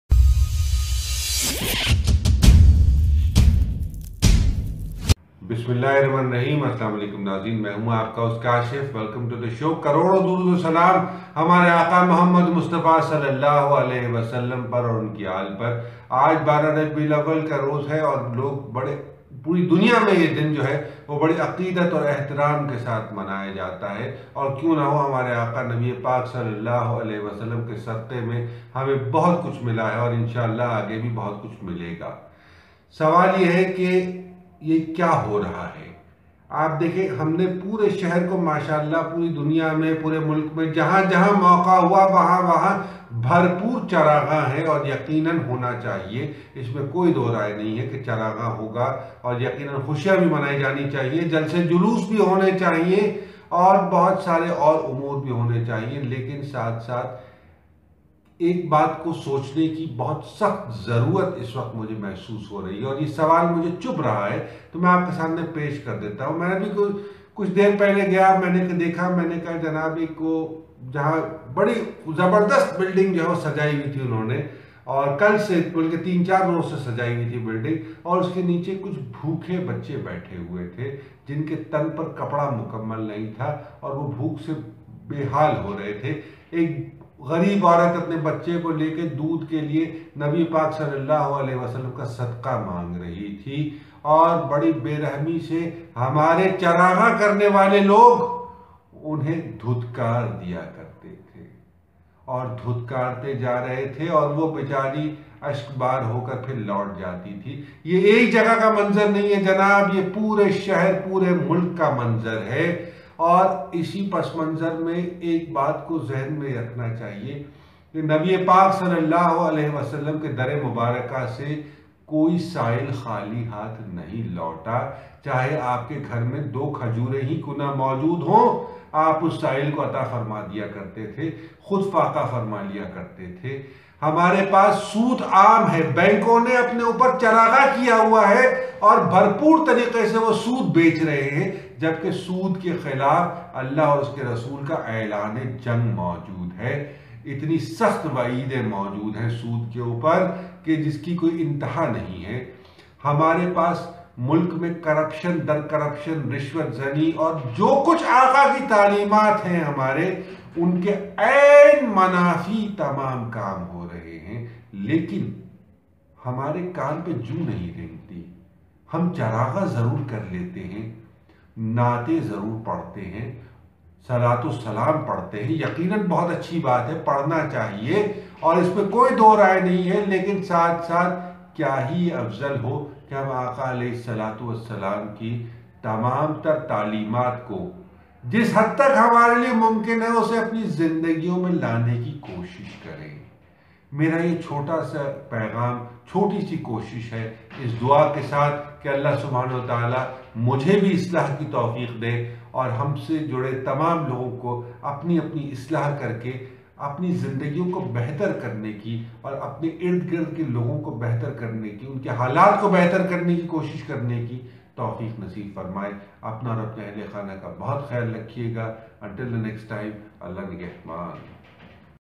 The Kashif Moten Show
Kia ab hamara Nabi Paak (SAW) se rishta sirf jashn, juloos o charaghan tak ka reh gaya hai?